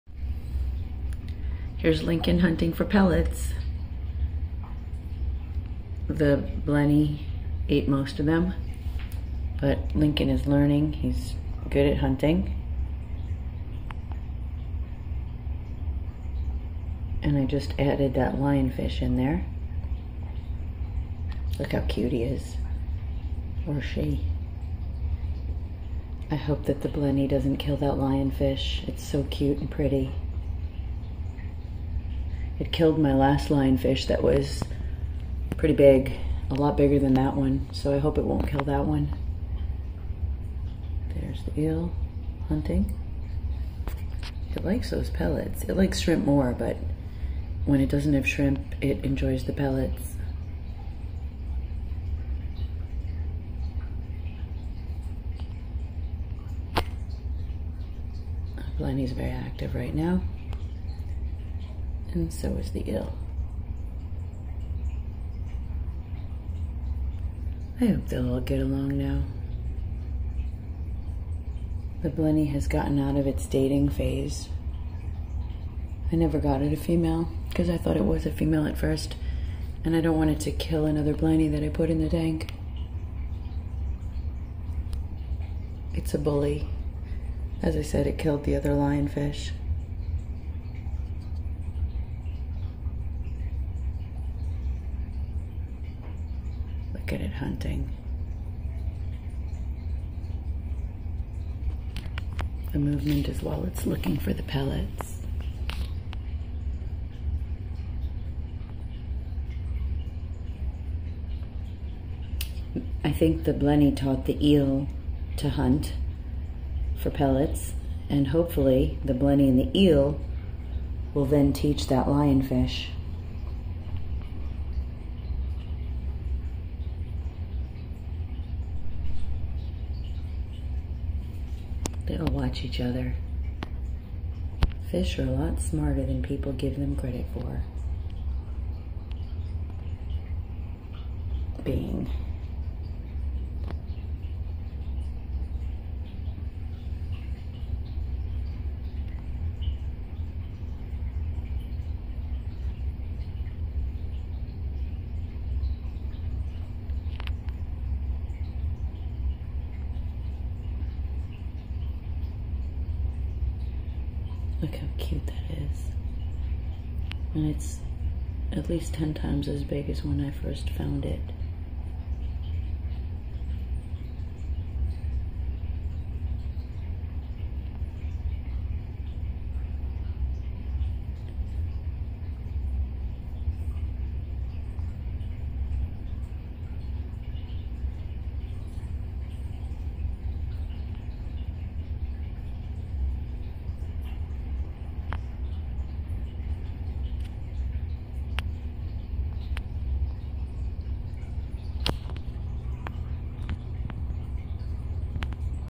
Baby spotted moray, male hairy blenny and new baby lionfish 7/21/22
The baby moray has gotten pretty good at hunting for pellets, the hairy blenny likes to dominate the tank, and I just added the baby lionfish. I'm hoping the blenny won't kill the lionfish like it did to the last, much larger lionfish.